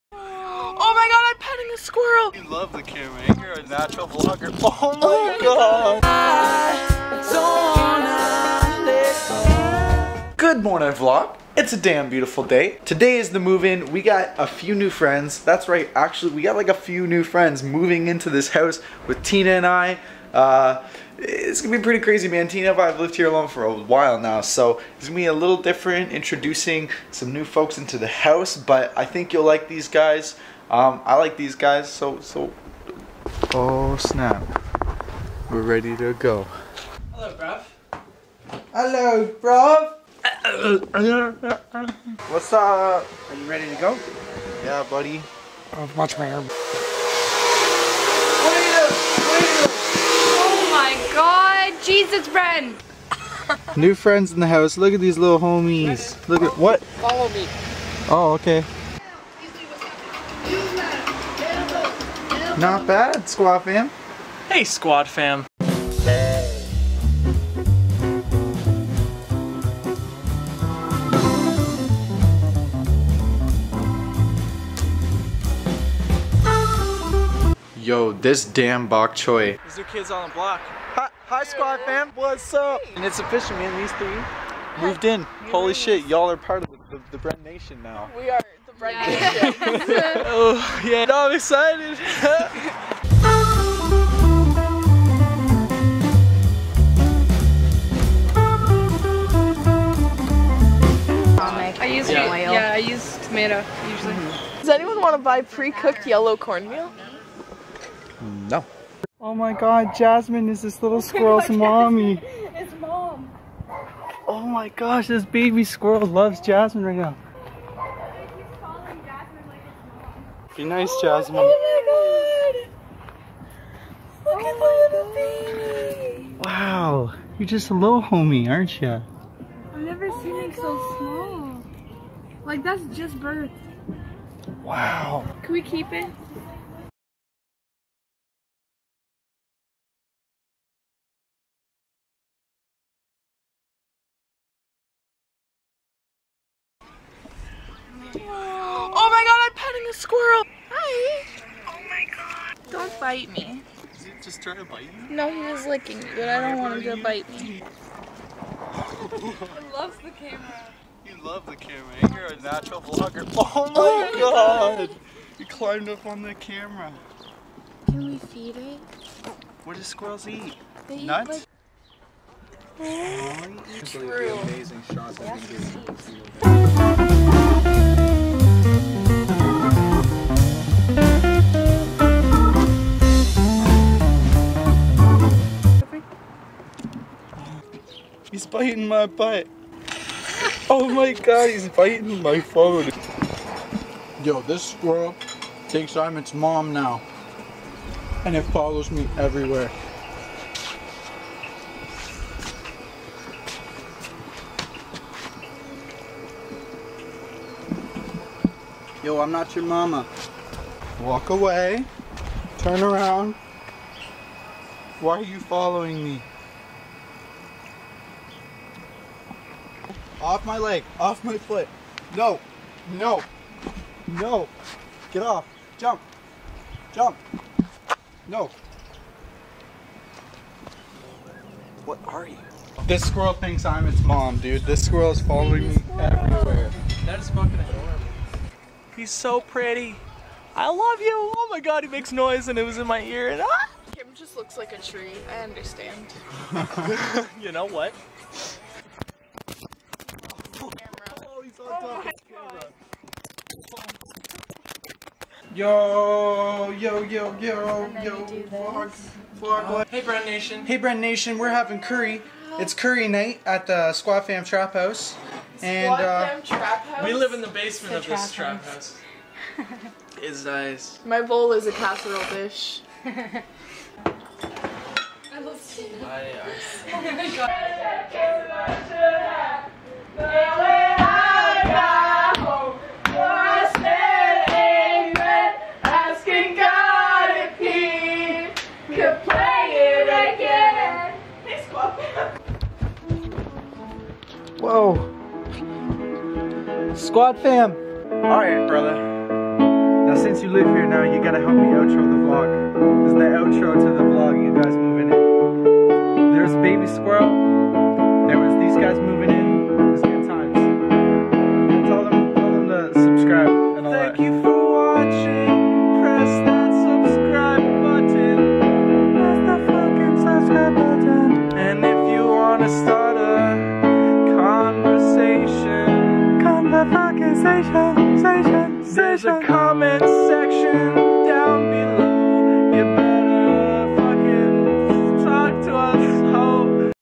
NEW ROOMMATES & A BABY SQUIRREL?
VLOG #292 - It's true we have 3 new roommates and a baby squirrel... well maybe not a squirrel, but at least we got mice!